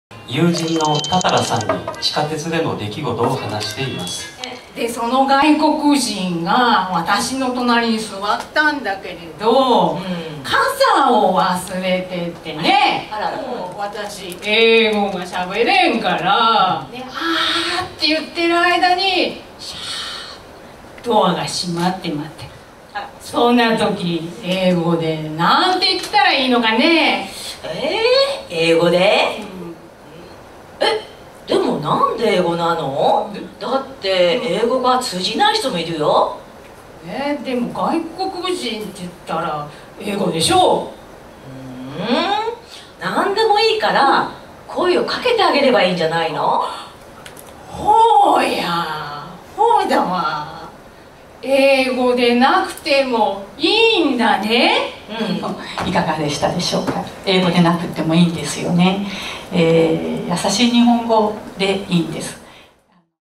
コントはやさしい日本語で 外国人との共生探る「やさしい日本語劇団」 名古屋
外国人が直面している難しい日本語や暮らしのルール、外国人との向き合い方までコント仕立てで伝える市民劇団が、名古屋で結成された。その名は「やさしい日本語劇団」。言葉を入り口に、外国人住民とのよりよい共生のあり方を考えている。
劇団結成のきっかけは昨年１２月、東海地方の日本語教室の講師やボランティアが集まったシンポジウムだった。テーマは「やさしい日本語」。有志がコントを演じたところ、好評を博した。「なくすのはもったいない」という声が上がり、劇団の形にしたという。一般の人だけでなく、外国人と接することがあるボランティアにも生かしてもらえるよう、研修会などでも披露する。